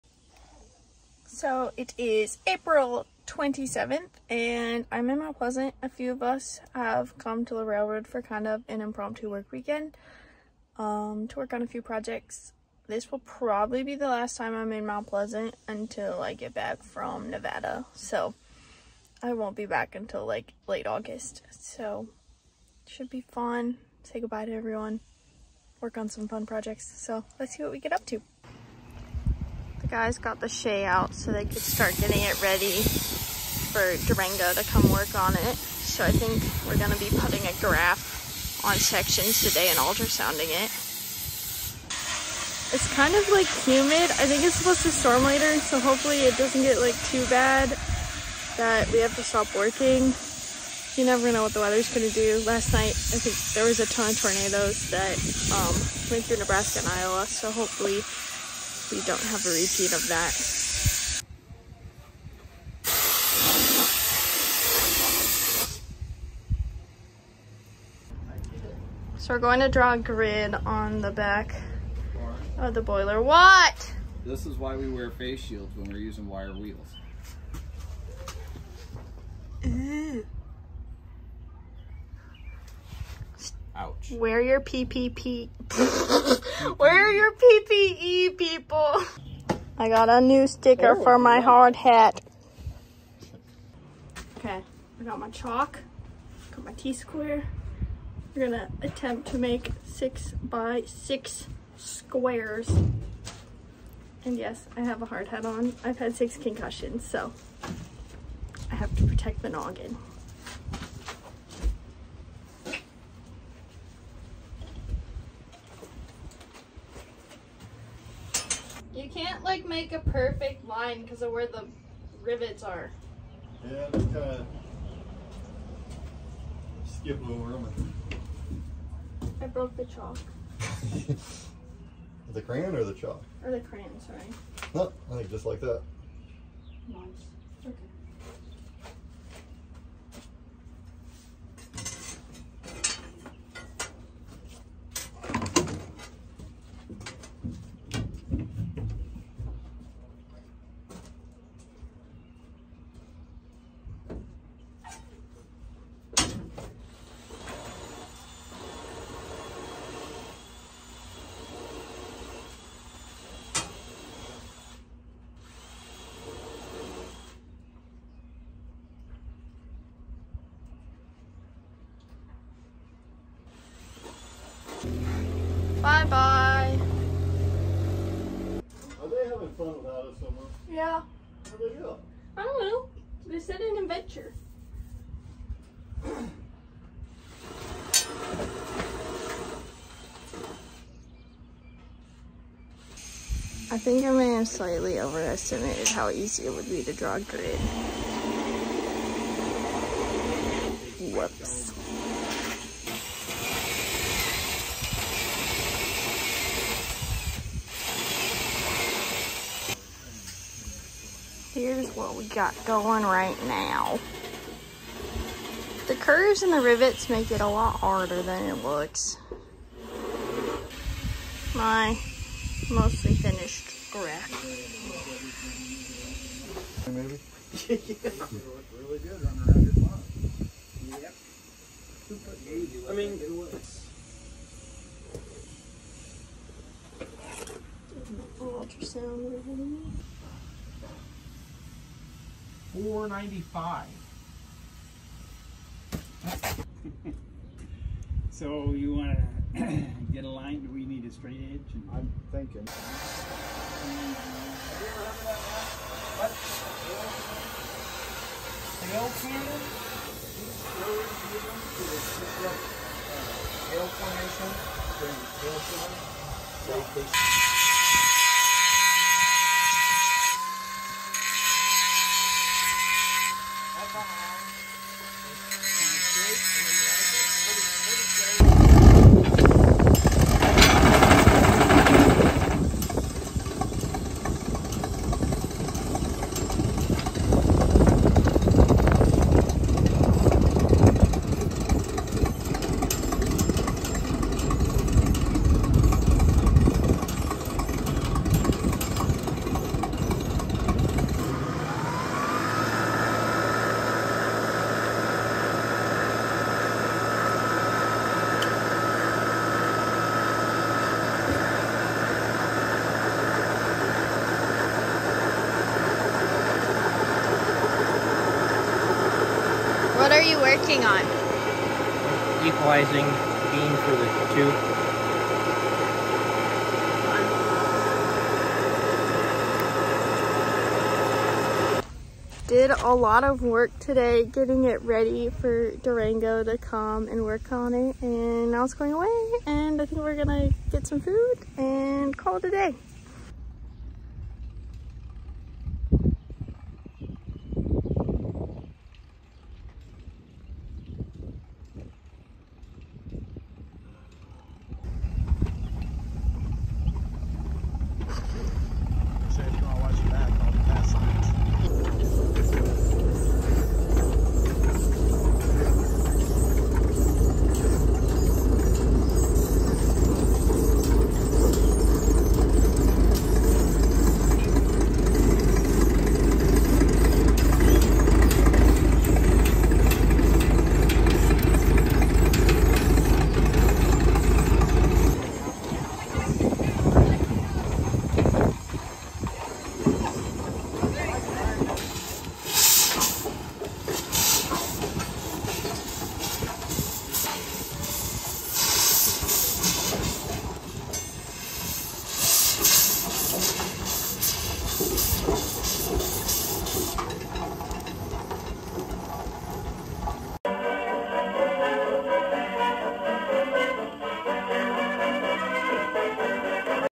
Railroad Work Projects - West Side Lumber Co 9 Shay, Boiler Graph, Cool Traction Engines
Went to Mount Plesant to help work on some projects with friends before I move for the summer. My main project was working on a boiler/ firebox graph for West Side Lumber Company No. 9. Midwest Central Railroad announced they are contracting Durango & Silverton for some firebox and boiler repairs. I definitely have some improvements I can make on graphs in the future!